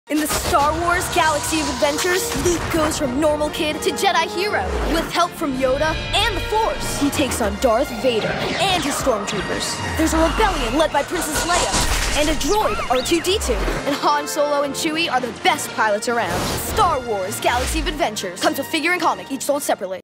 Star Wars Galaxy of Adventures Figures and Mini Comics
Star Wars fans new and old can discover the exciting action from the galaxy far, far away with figures, Lightsabers, and more from Star Wars Galaxy of Adventures! Learn about the most iconic characters from the Star Wars movies and read the included mini comic or scan the included QR code to join in on the adventures of fan favorites like Darth Vader, Han Solo, Princess Leia, Luke Skywalker, Chewbacca, and more! With Star Wars Galaxy of Adventures, the galaxy far, far away comes home!
Each pack contains 1 3.75-inch figure, 1 mini comic, and 1 QR code that kids can activate with a smart device to discover more stories online about the character! May the Force be with you!

Star Wars products are produced by Hasbro under license from Lucasfilm Ltd.